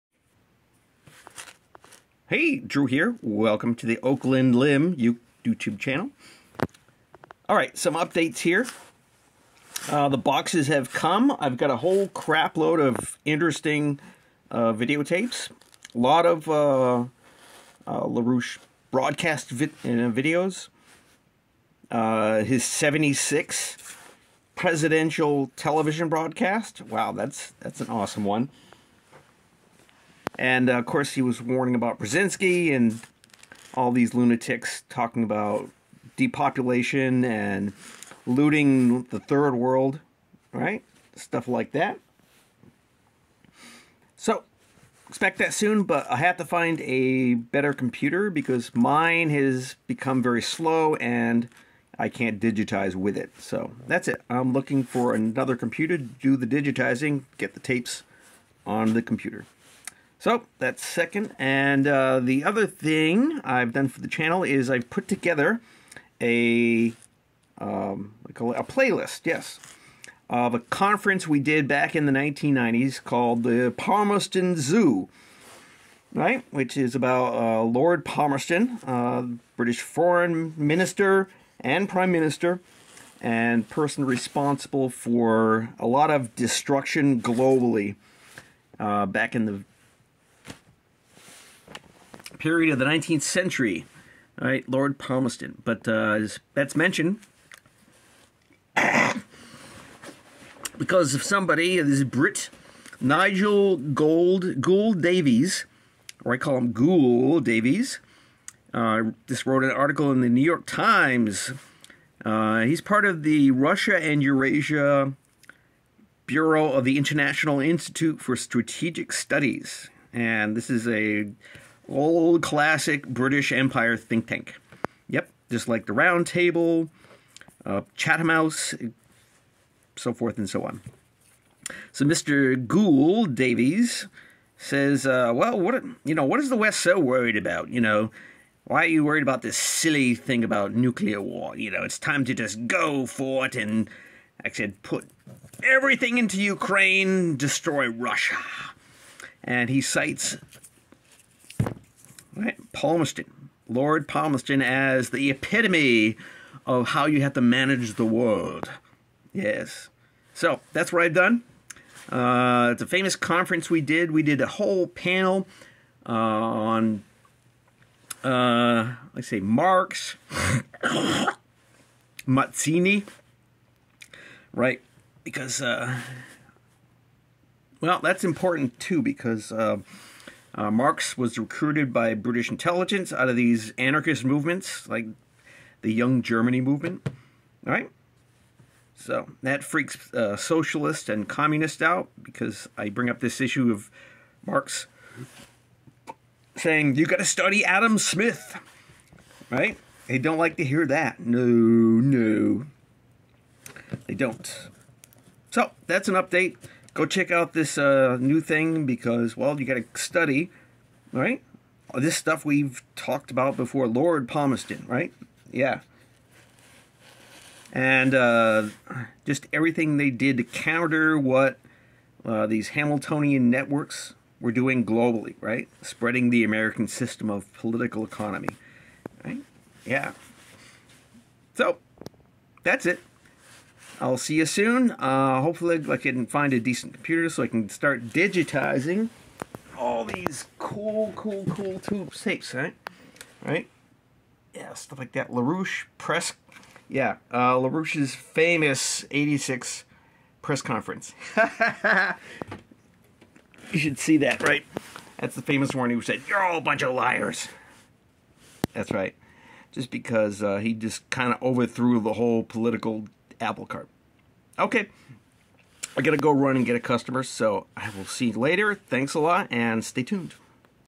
New Playlist
In response to a Brit from the IISS expressing his love for former PM Lord Palmerston's style of managing the British Empire.